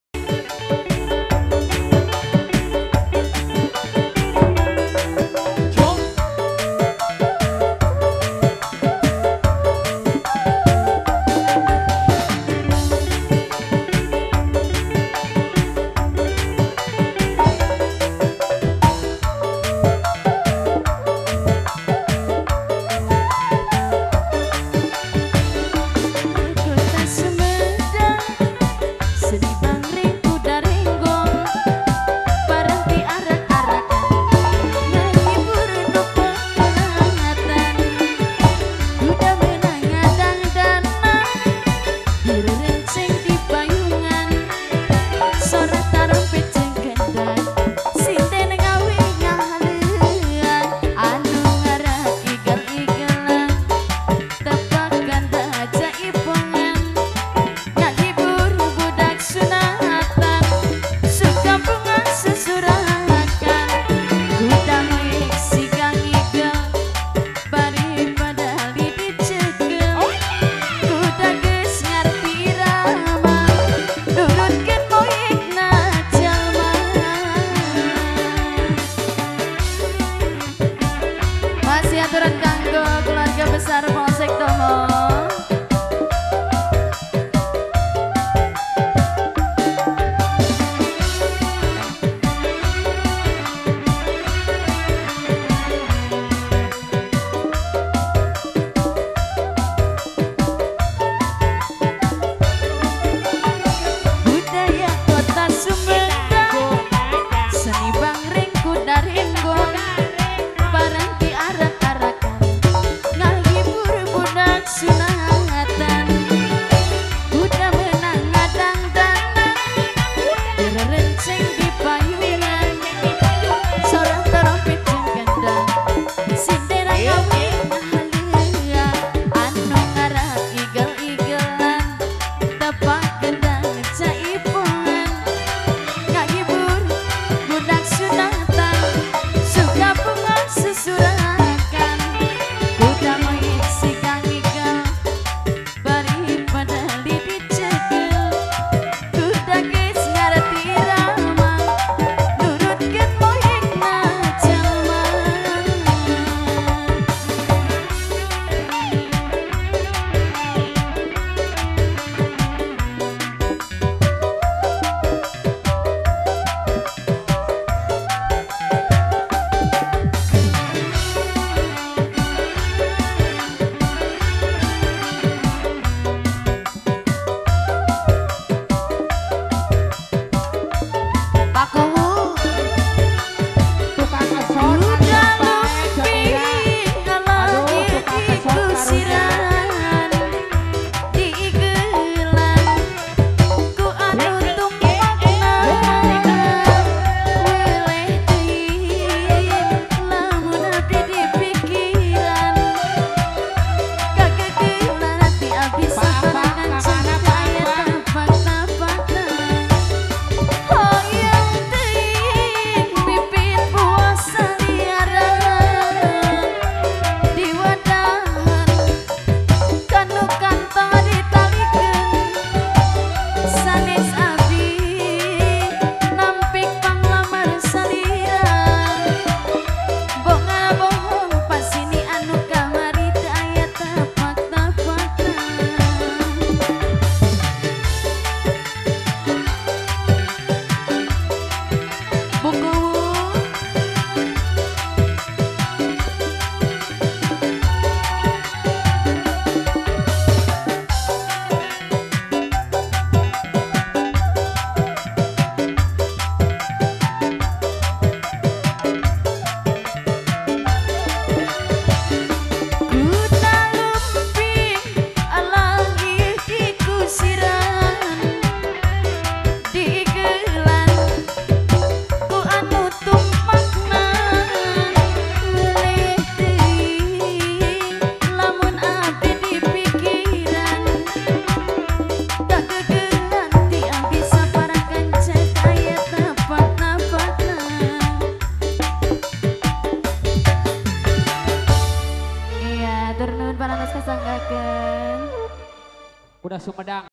34 RIMBA NADA ENTERTAINMENT @ Darmawangi Tomo Sumedang
Darmawangi, 24 Oktober 2019

Khitanan
Aqilah Syabil Muhammad Haidar

Keluarga Hajat
Bpk. Dadang Kurniwan
Ibu Nining Widaningsih

Alamat
Desa Darmawangi RT. 03/01
Kec. Tomo Kab. Sumedang
ORGAN TUNGGAL RIMBA NADA
Pimp. Dadang Kurniawan Darmawangi Tomo Sumedang
AR Audio System